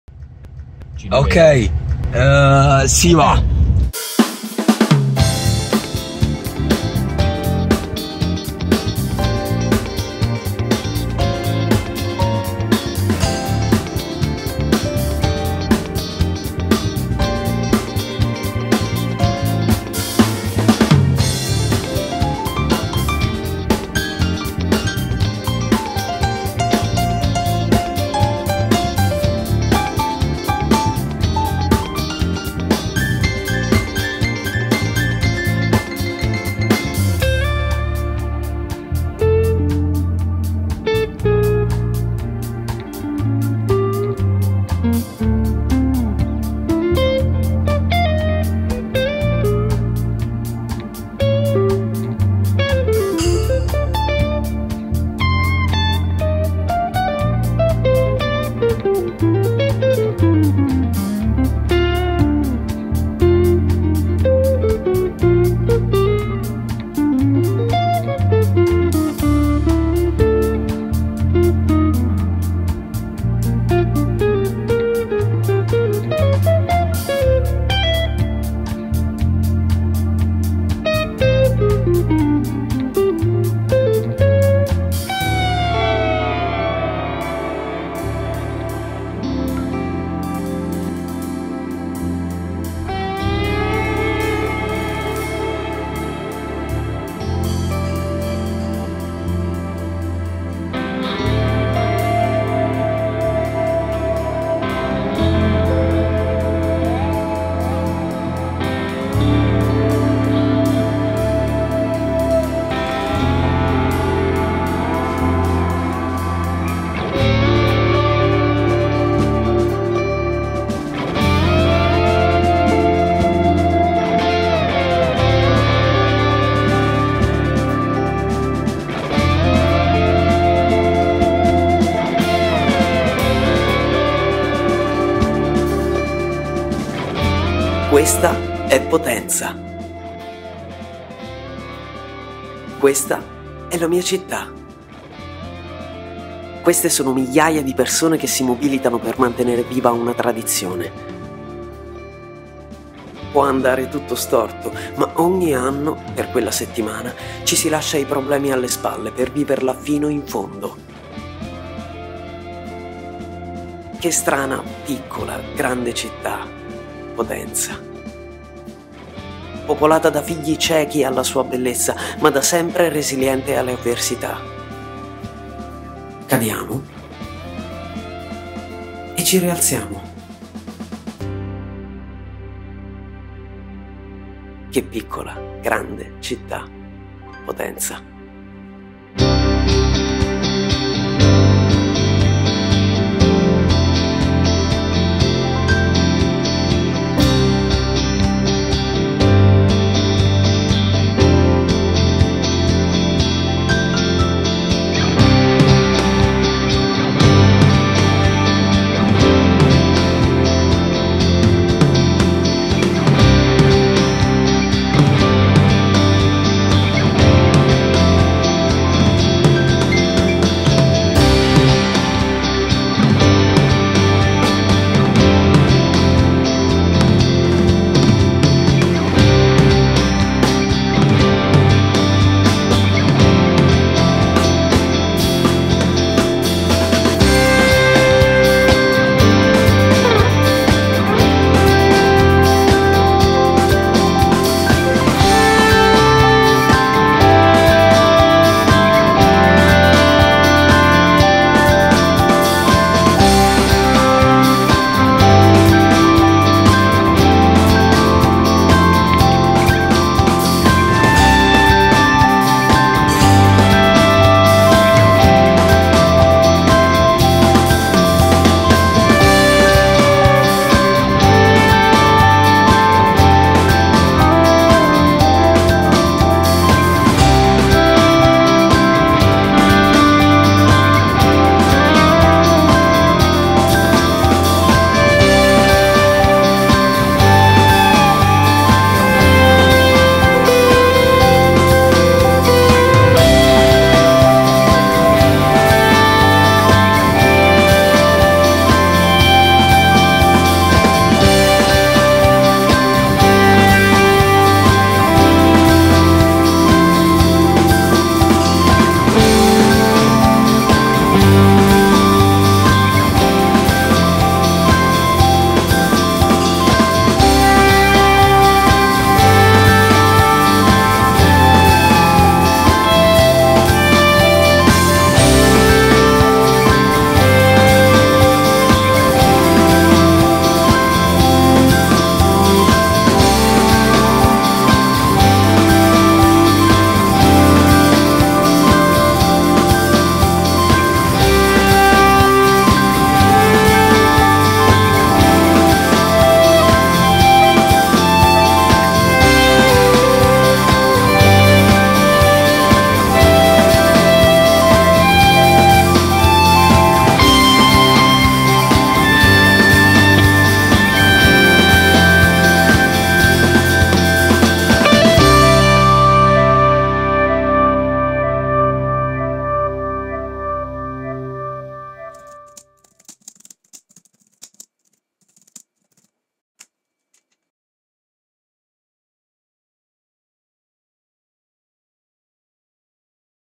Una piccola grande città!
Con quasi due mesi di ritardo (colpa di quel male incontrastabile chiamato 'sessione estiva') vi presentiamo finalmente il nostro video sulla settimana di S.Gerardo! In quei giorni spensierati, come forse alcuni di voi avranno visto, abbiamo girato armati di reflex e futuristici supporti per le riprese da smartphone, cercando di catturare il nostro punto di vista sull'allegria e l'aggregazione della festa. Questo che vi mostriamo è il risultato, ci scusiamo in anticipo per avervi reso un po' tutti attori inconsapevoli del nostro video e speriamo vi piaccia! Ci teniamo, inoltre, a ringraziare il Comune di Potenza per i pass concessici, il Cantiere e Hops per averci permesso di fare le riprese.
Buona visione!

Montaggio e postproduzione: Francesco Avigliano

Fotografia: Antonio Nolè

Voce: Alessandro Freschi

Mix e Mastering: Gerardo Grande

Musiche composte da: Mirko Mecca

Musicisti: Mirko Mecca - piano
                  Gerardo Grande - chitarra
                  Andrea Pergola -  batteria
                  Antonio Nolè - basso
Potete seguirci su Instagram

O su Facebook